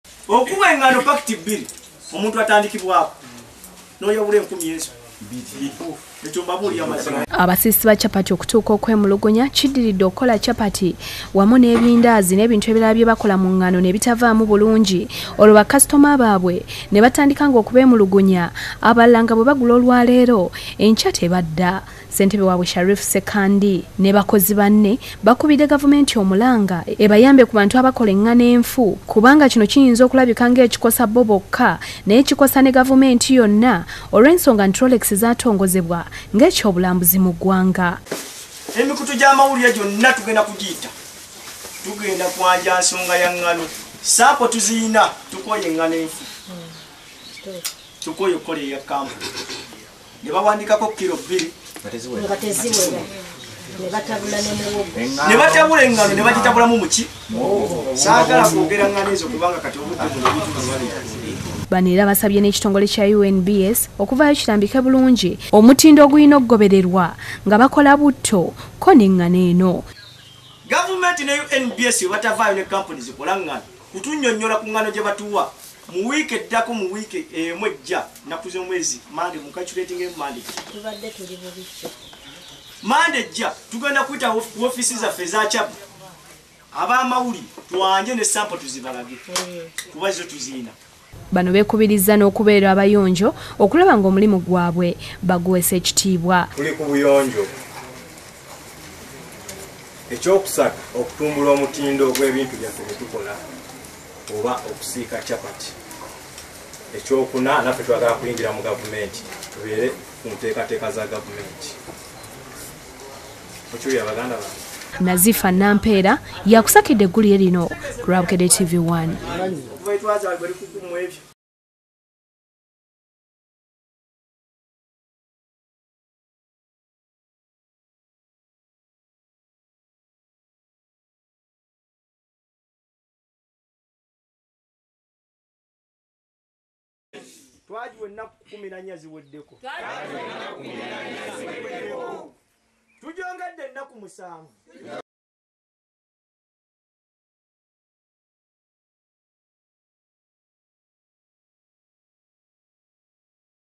Abasiisi ba 'Chapati' beemulugunya
Bakukuba abakola engano mu mbuga.